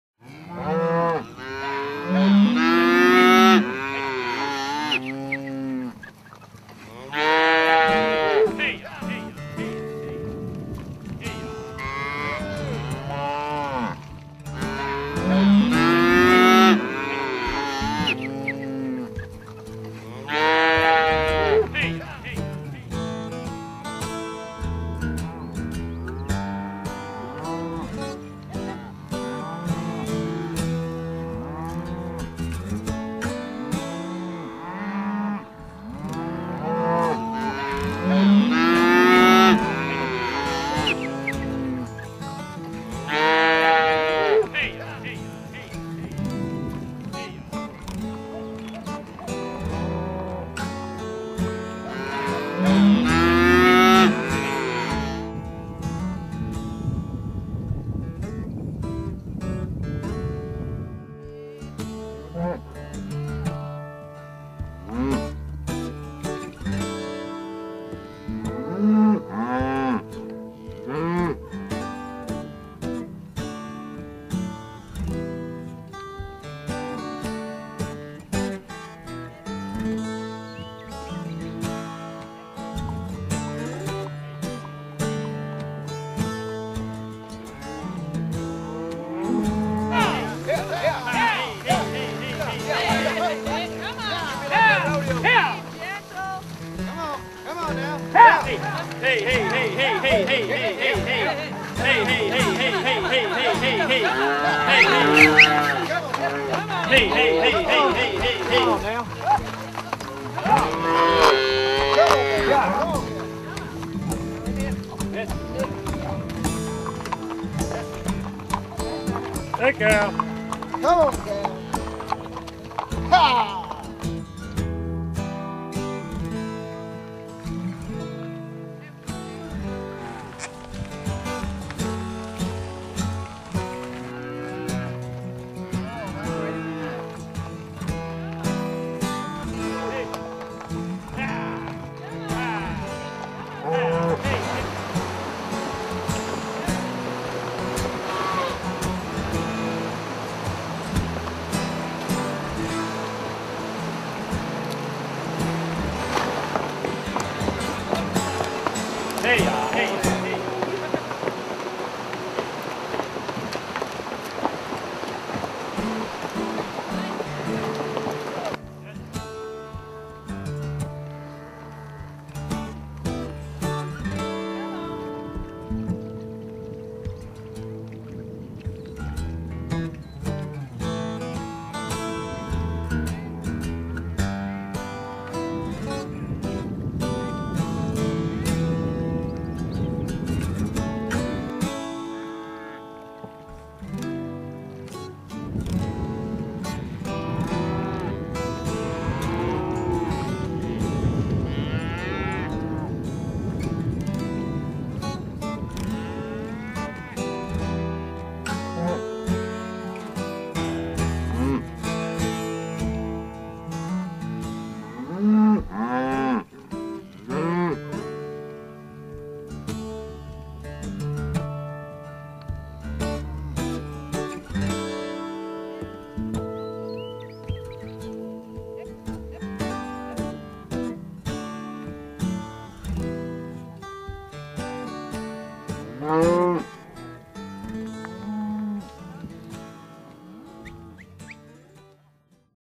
Cattle Drive at the Laughing Water Ranch
Just like in the old west, stay for a week, get to know your horse, learn how to herd, and Friday drive cattle over 10 miles to a grazing pasture. What a vacation!!!